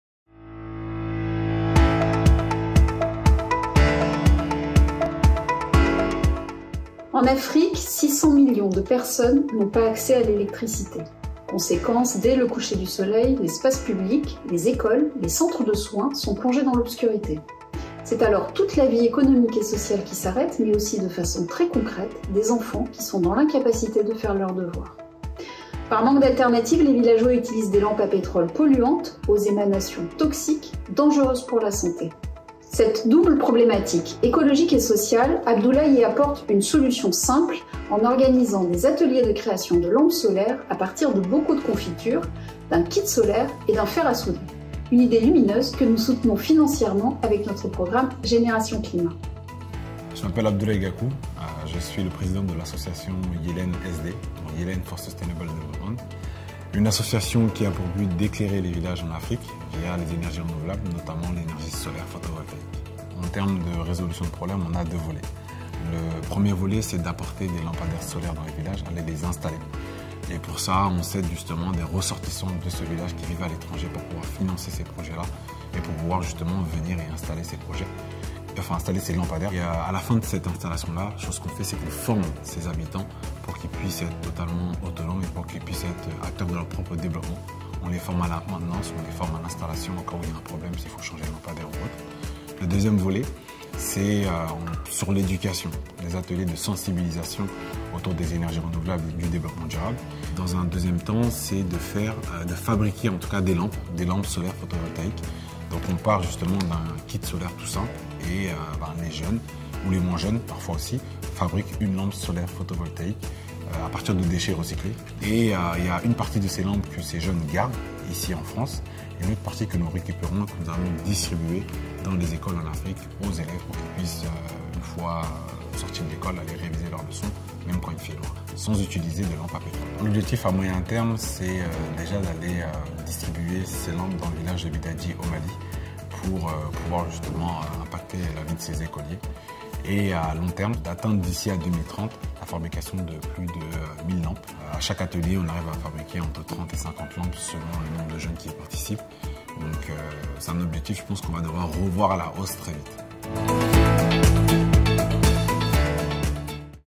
☀️ Des lampes solaires pour éclairer l'avenir des écoliers maliens
🔌 En Afrique, 600 millions de personnes n’ont pas accès à l’électricité. Pour aider les populations des zones rurales isolées à s’éclairer grâce à l’énergie solaire, Abdoulaye, ingénieur de formation et enseignant de technologie en région parisienne a créé en 2019 Yeelen for Sustainable Development.

💡 A travers notre programme Génération Climat, nous soutenons l’action inédite de cette association qui organise des ateliers de formation où, à partir de déchets recyclés, des jeunes français fabriquent des lampes solaires à destination des écoliers au Mali.